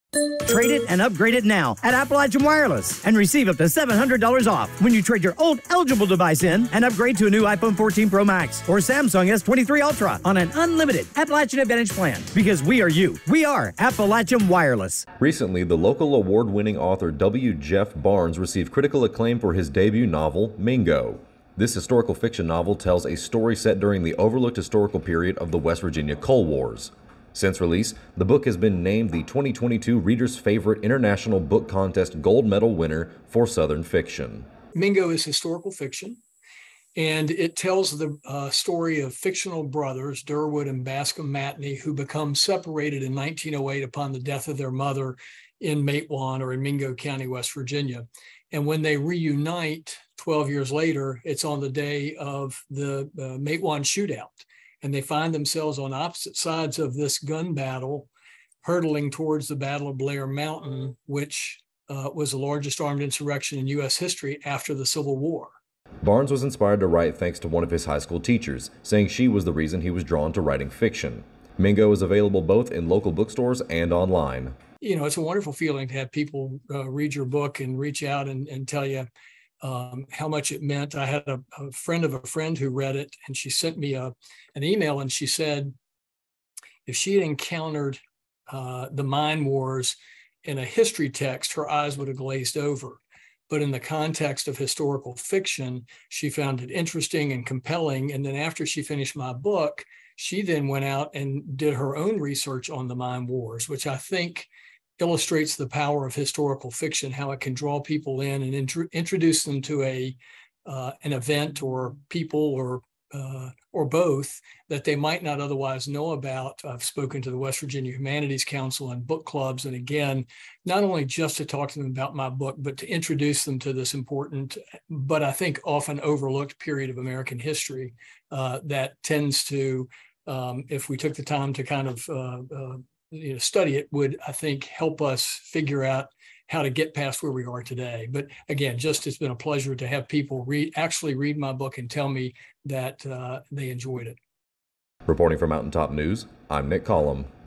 Local Author Receives Acclaim for His New Historical Fiction Novel
Local author W. Jeff Barnes has recently received critical acclaim for his new historical fiction novel.